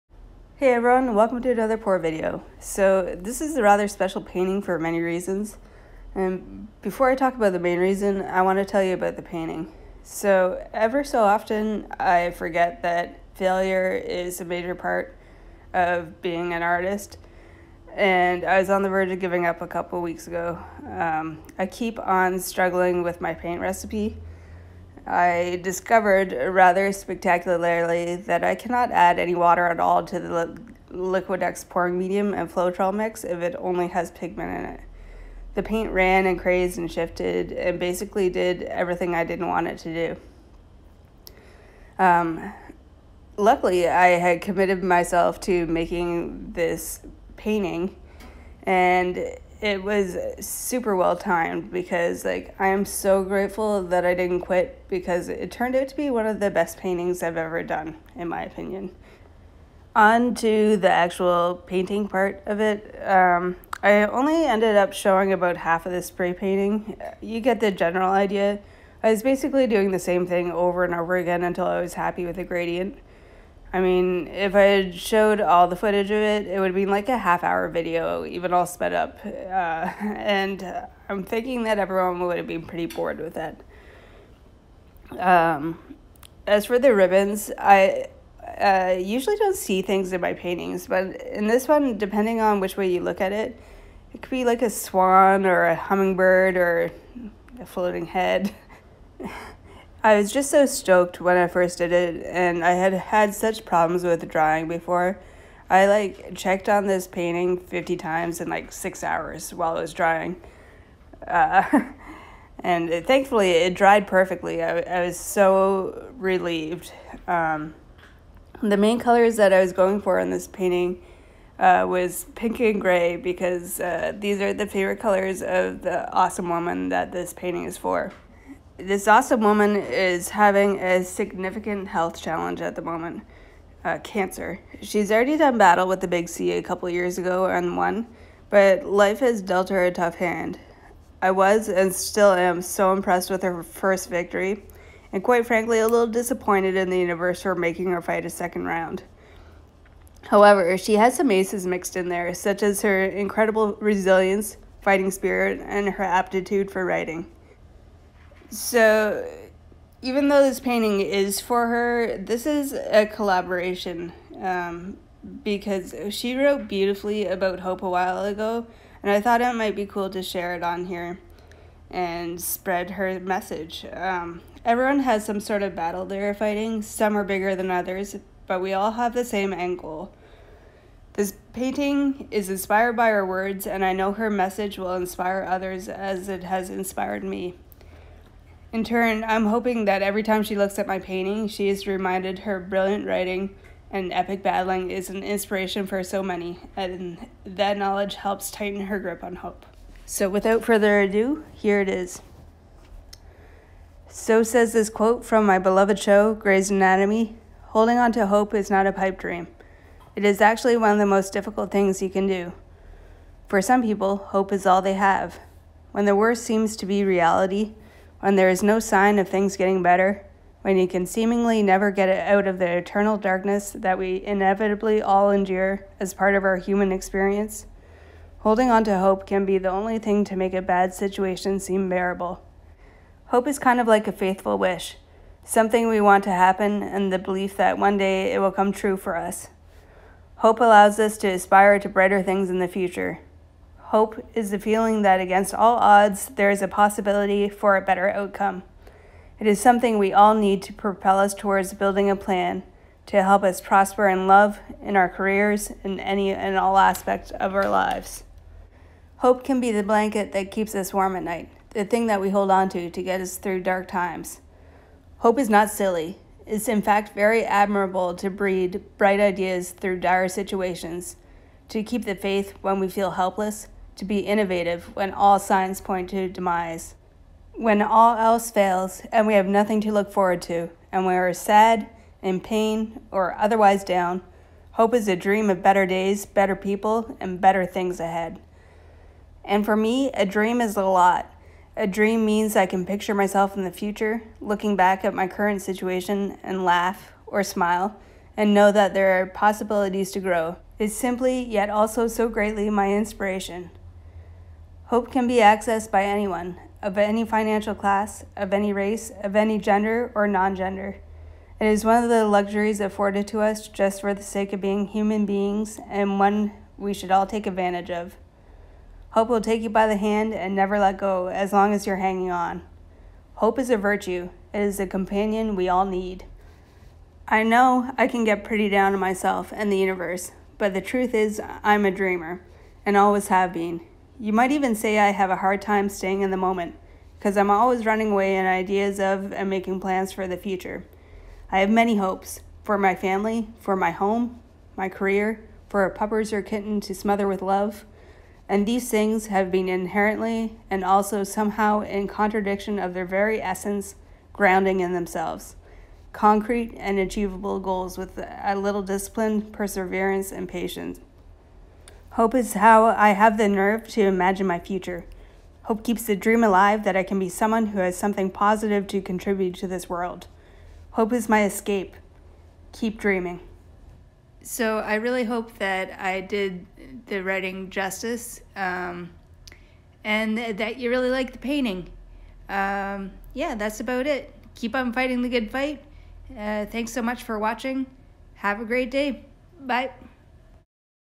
“Hope” Acrylic paint pour with a ribbon
This painting is for an awesome woman who wrote what I read in the middle of this video-it is all about hope and inspiration. First collaboration ever, and I hope my reading of her epic writing did her justice! The ribbon is one of my best ever thank goodness!